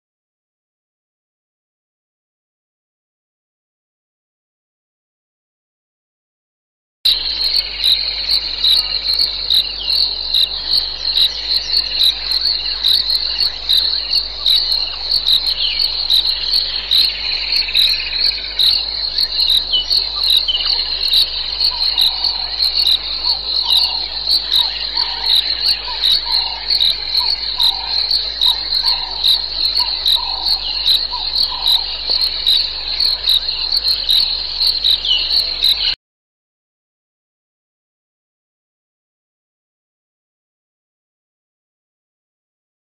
Water monitor on Tembe 11 Jan 2012.wmv
Water monitor making it's way in the foreground